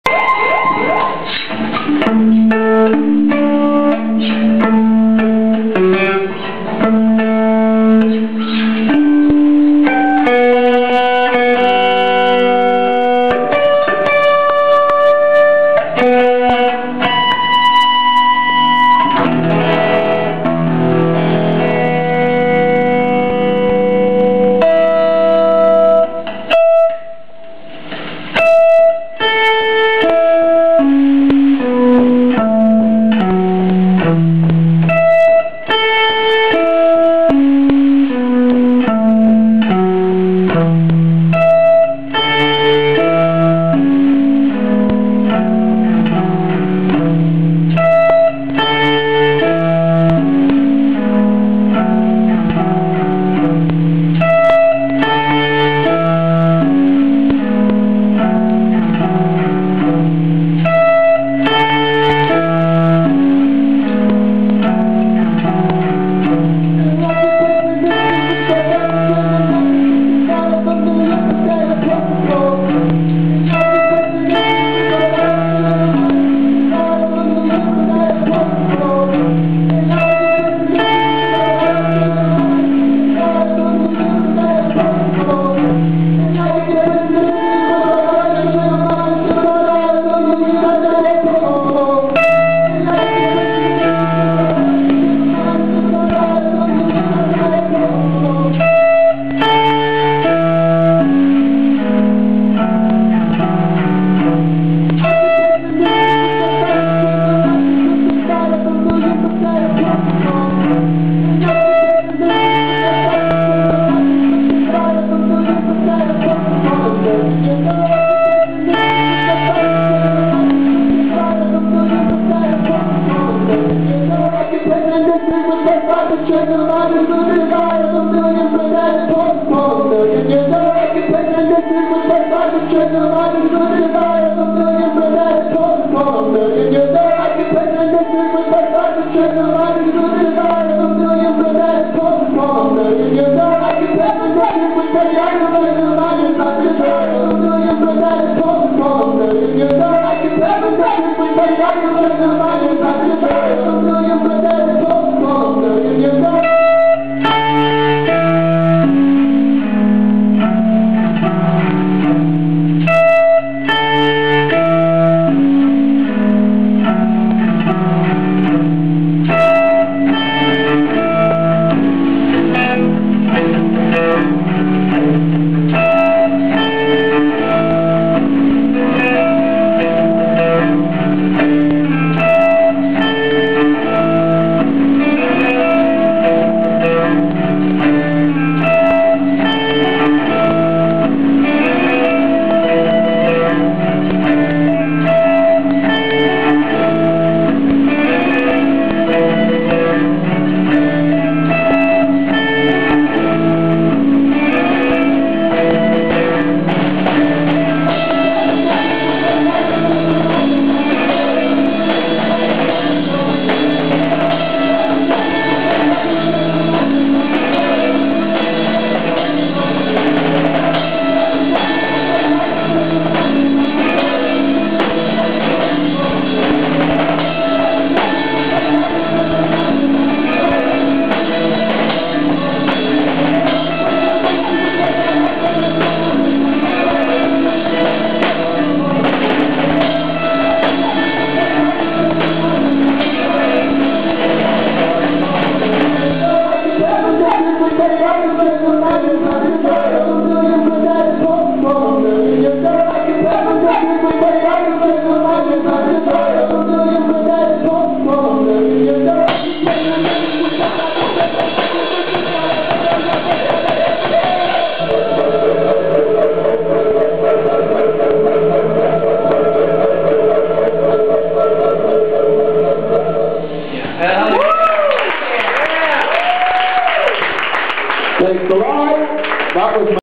Dugout Canoe - unknown - September 29, 2008
This is Dugout Canoe performing at Rhinoceropolis. Sorta dark.